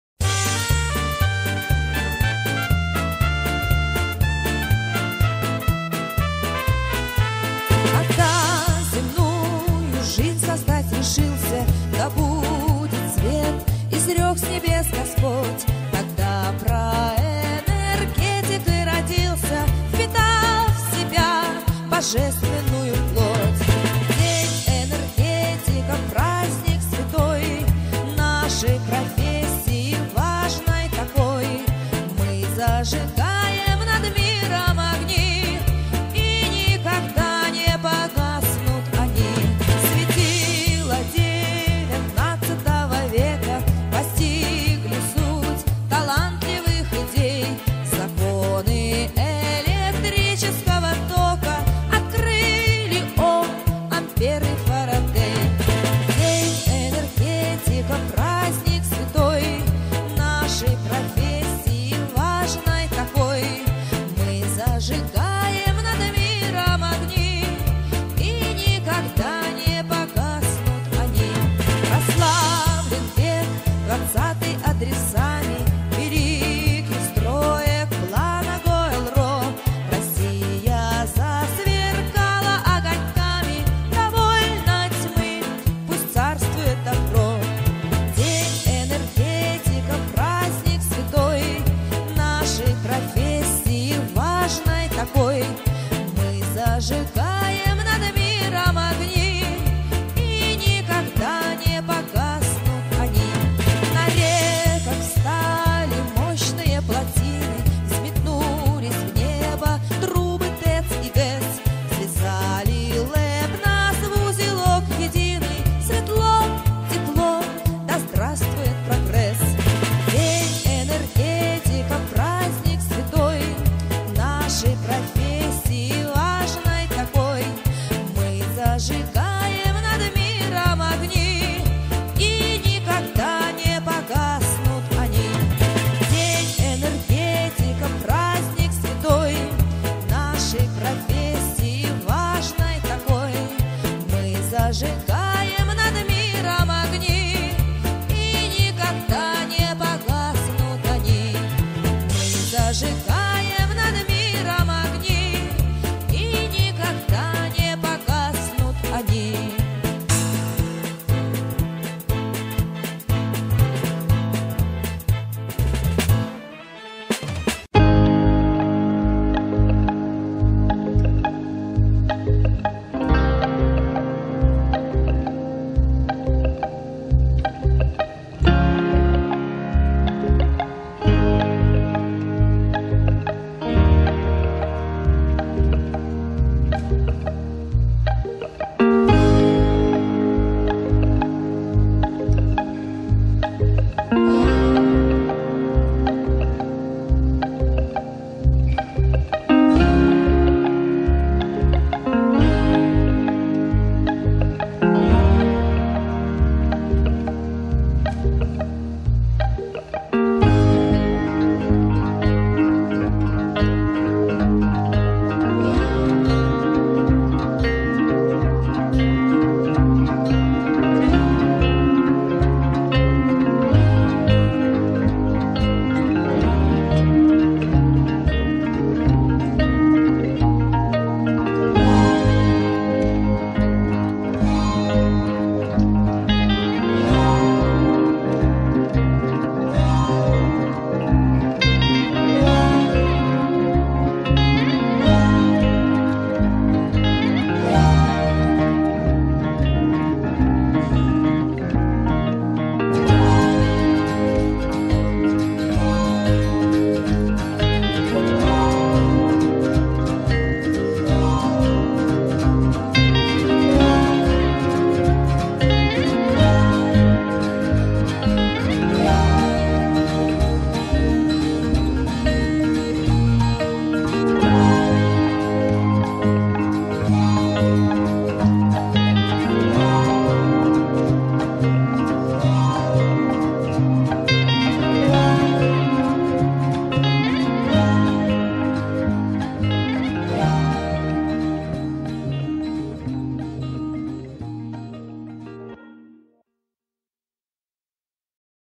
45 лет Северодвинской ТЭЦ-2. 22.12.2021.Как было и как стало. С Днём Энергетика!
Строительство ТЭЦ-2 в Северодвинске начали в 1972-3 годах на болоте. А пуск был в декабре 1976, первые мегаватты. Первый вид топлива был мазут. Сейчас это запасной вид топлива. ТЭЦ-2 переведена на природный газ. С Днём Энергетика!